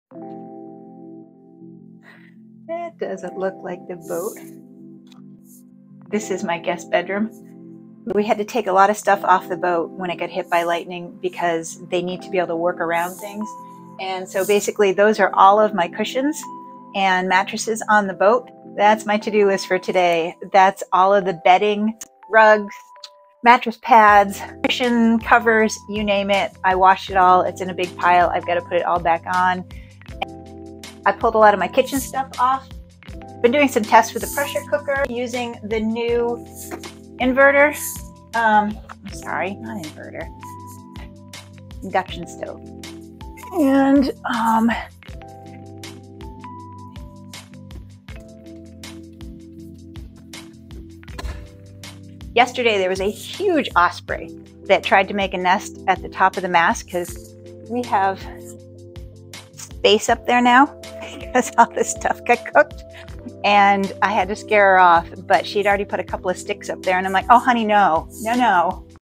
The one where I had to pull a lot of stuff off the Reboot
With lightning repairs about to commence, I had to take a lot of stuff off the boat to make it easier for them to repair it.